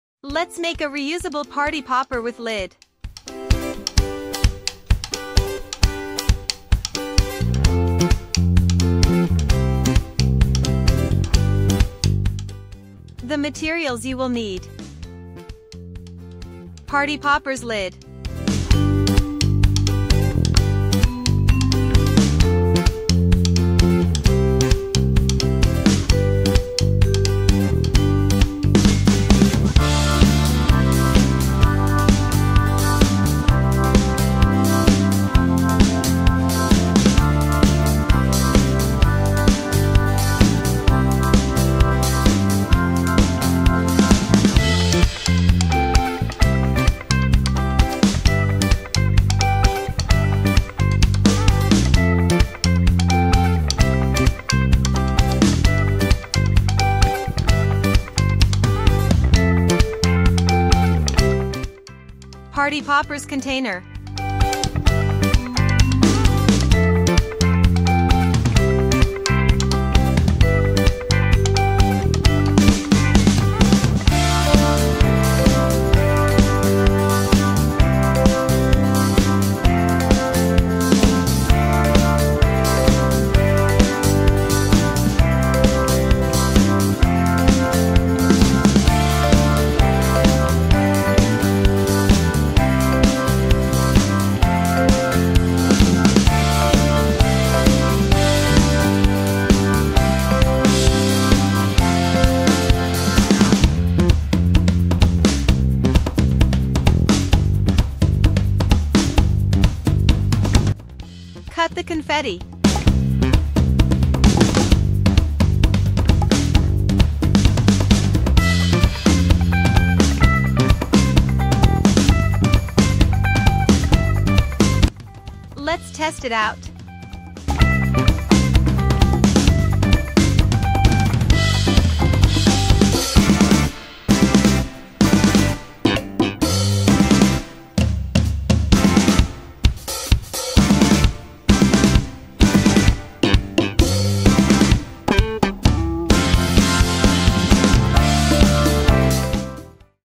REUSABLE Party Popper Tutorial | DIY Confetti Popper using toilet paper roll crafts
It all started when I wanted to make a DIY party popper without using balloons. Now there are a few more other ways to make a party popper at home. You can keep reusing this party popper for endless fun. Bonus, make the lid to make sure the confetti will not fall out while not in use.

Tag me on Instagram @joyincraft if you have made this! xo